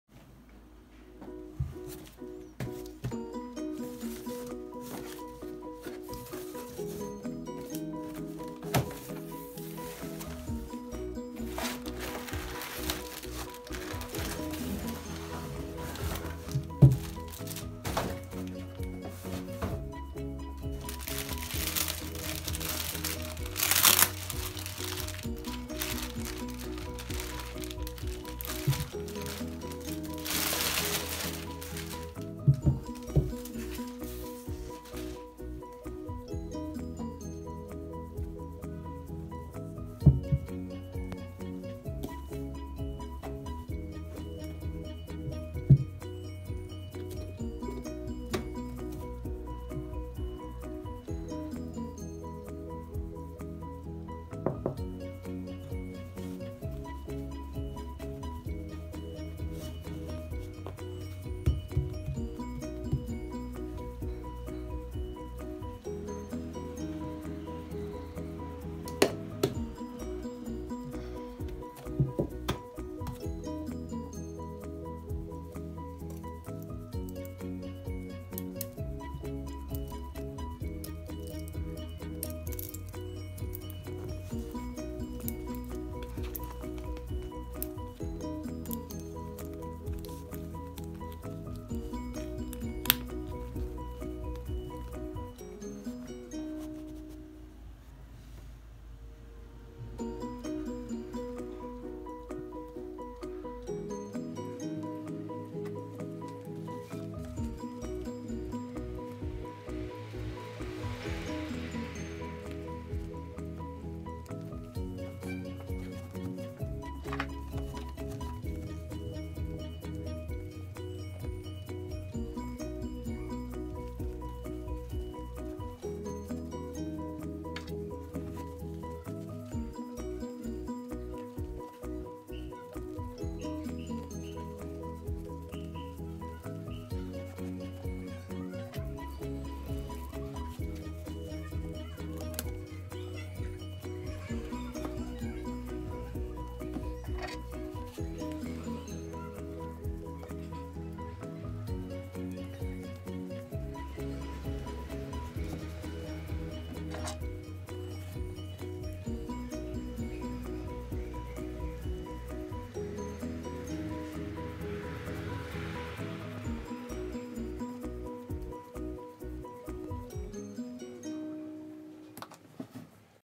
CHAVIN MARKA 15'Lİ SAAT KUTUSU | KUTU AÇILIMI
Chavin marka saat kutusu 15 adet saat alımına uygundur. Fiyat kalite performansı son derece iyi ve tavsiye edilir.